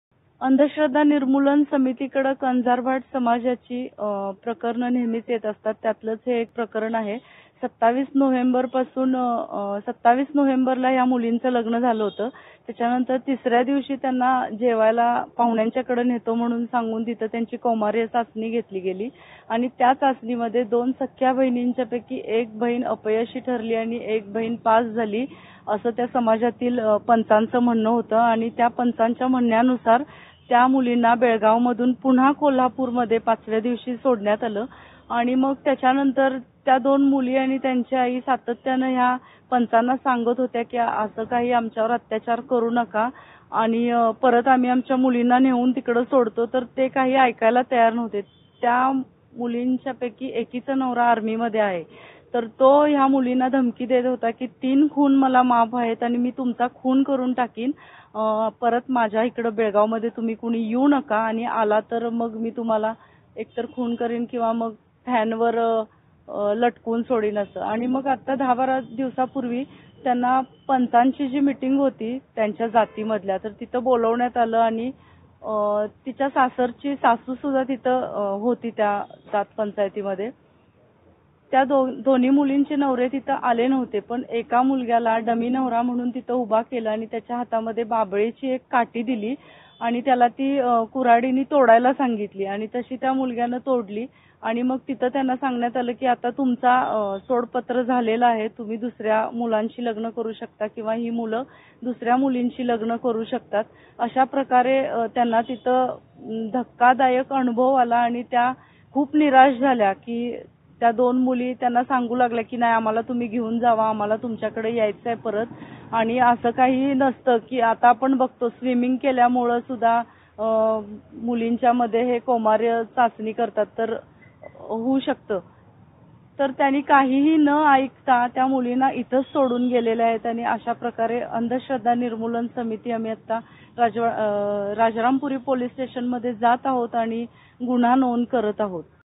Family in Belgaum persecuted two sisters in Kohlapur |ETV Bharat
कोल्हापुरातील दोन सख्ख्या बहिणींची लग्नानंतर कौमार्य परीक्षा घेतल्यानंतर एक बहीण अपयशी ठरल्याने तिला माहेरी पाठवल्याची धक्कादायक घटना घडली आहे. कोल्हापूर येथे कंजारभाट समाजात दोन सख्ख्या बहिणींची लग्नानंतर कौमार्य परीक्षा घेतली असता त्यातील एक बहीण कौमार्य परीक्षेत फेल गेल्यामुळे बेळगावमधील सासरच्या लोकांनी या दोघींनाही नांदवण्यास नकार दिला असून त्यांना त्यांच्या घरी पाठवून देण्यात आले.